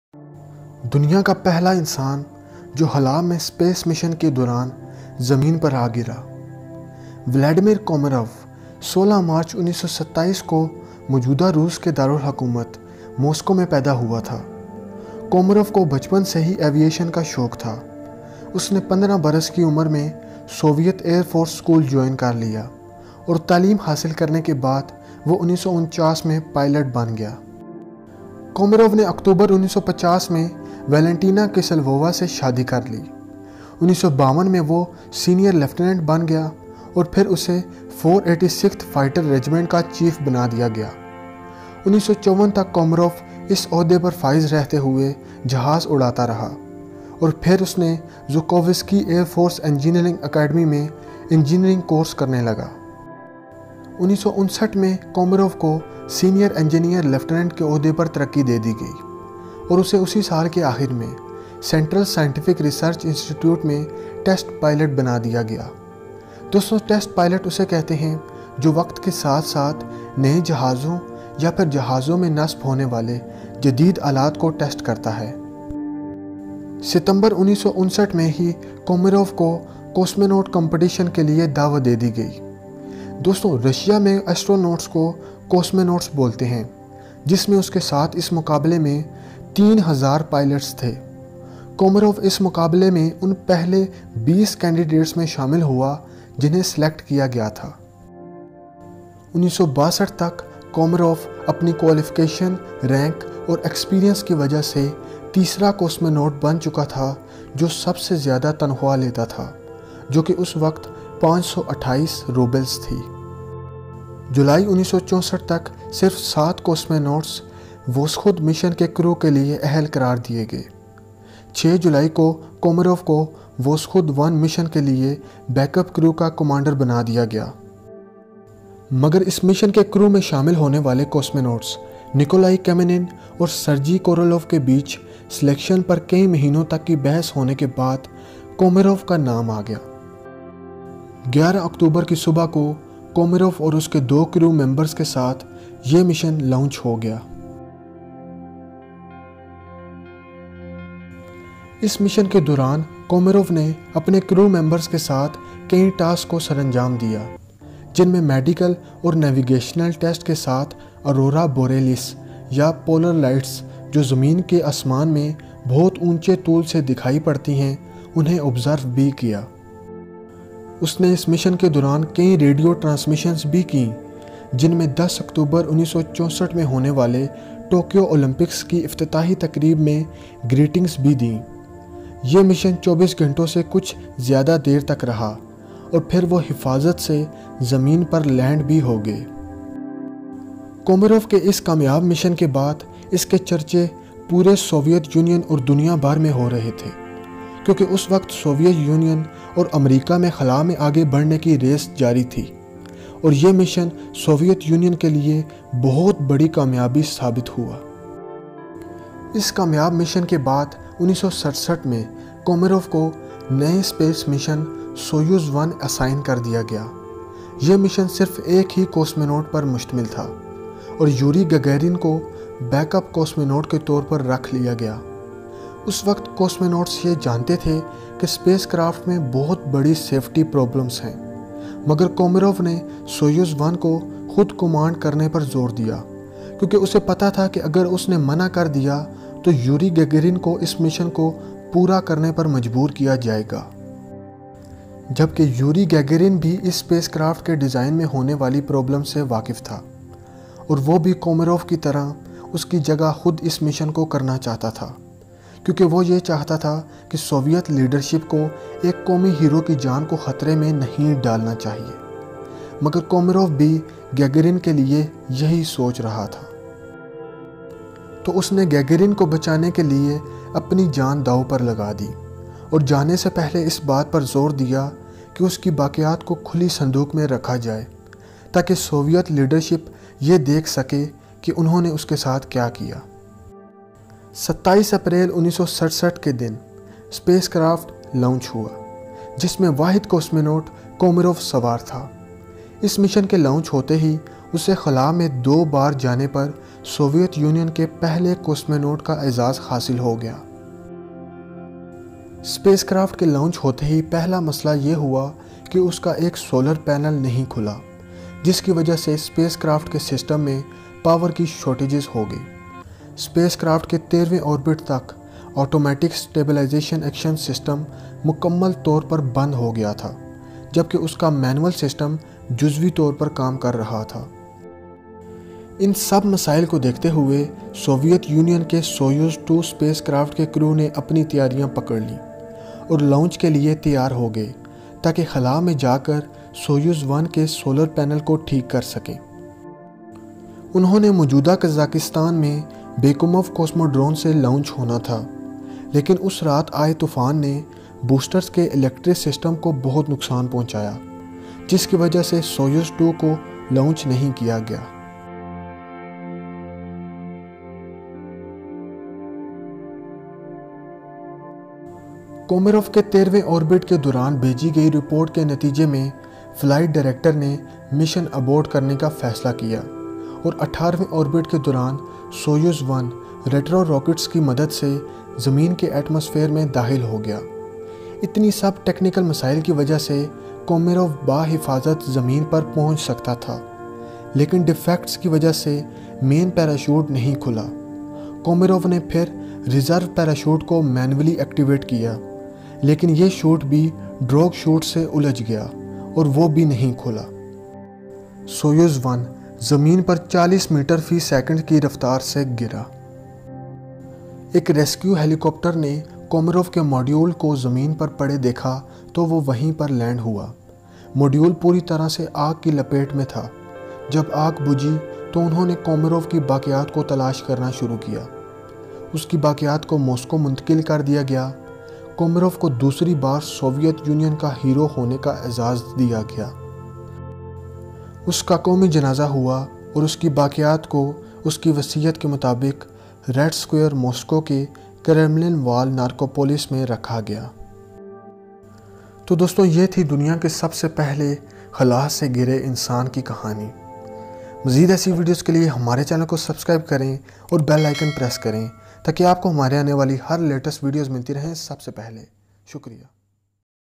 Biography #09 | Who was Happend to Vladimir Koramov? | First Man who fells from Space | Urdu
Vladimir komarov was a soviet cosmanaut born in moscow. After he became fight pilot and joined the competition for cosmanaut. he became chief of voskhod 1 spacecraft crew in 1964. and after his successful misson. he has assigned soyuz 1. Cosmanauts knows the problems and errors in spacecraft but he pursue to do it to save the life of cosmanaut yuri gagarin the backup crew of soyuz 1. 27 april 1967 komarov launched. from the begining his spacecraft's one solar panel did'nt open which causes shortage of power. in his 13 orbit automatic stablizing action system went completly shut down. in his 18 orbit he was order to abort mission . komarov started the retro rockets but his parachute did'nt opened and he falls at the speed of 40 m/s. he crashed in kazakhstan. his remains were send back to moscow where he has given a national funeral.